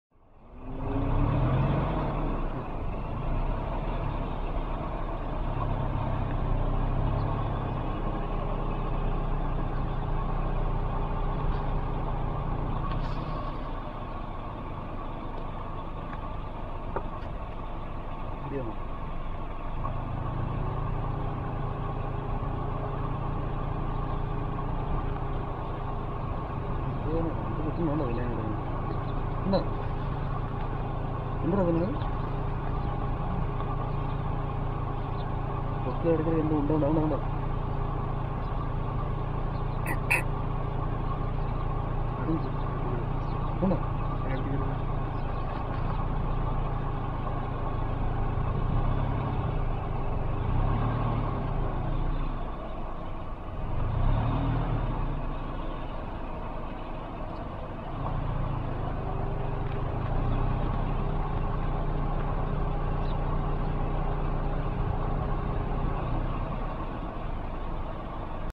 Hudhud Bird finding prey,Oman
Hudhud, the bird after which Oman named the cyclone